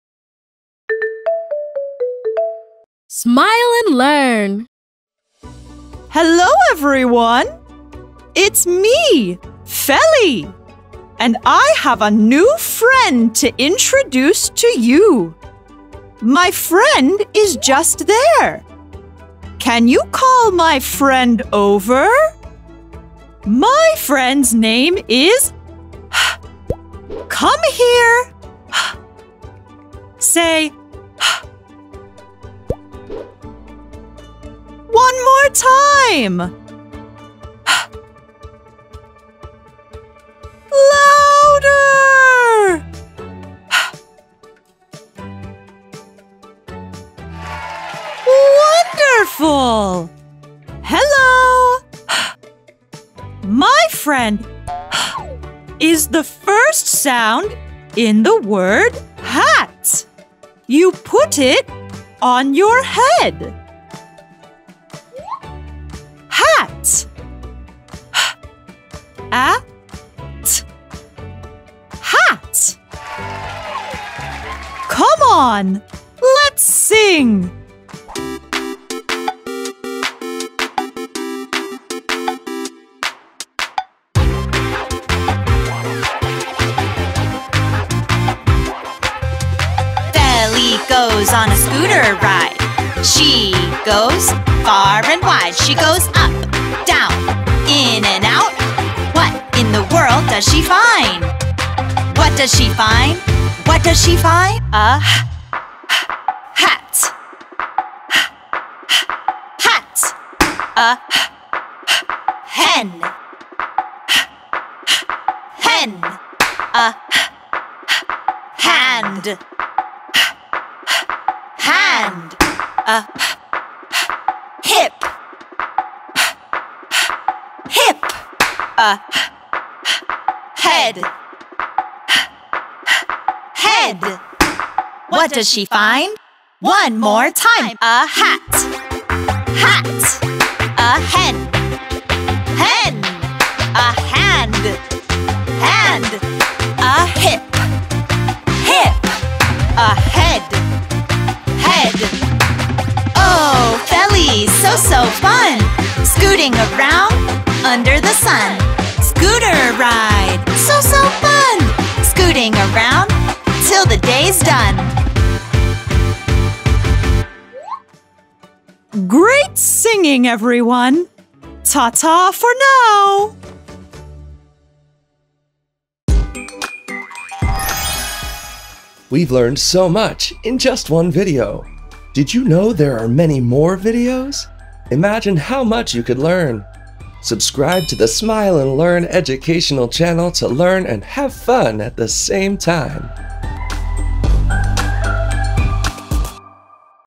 Phonics for Kids 🗣 The /h/ Sound 🖐 Phonics in English 🐓
Educational video for children in which children will learn the /h/ sound in English. Little ones will discover how to pronounce  the sound of the letter H through a fun song and numerous examples alongside our favorite seal, Feli. Phonics is a very successful method used to teach reading through letter sounds. In this simple video for children who are beginning to read, we work on phonemic awareness and phonological awareness, two important skills for early readers and young children who are developing reading skills. It is an interactive video that asks viewers to participate by singing and repeating the sounds along with Feli.

This video is a very useful and interesting resource for children to learn the sounds of the English language. It is an excellent video for preschool, primary, and bilingual education.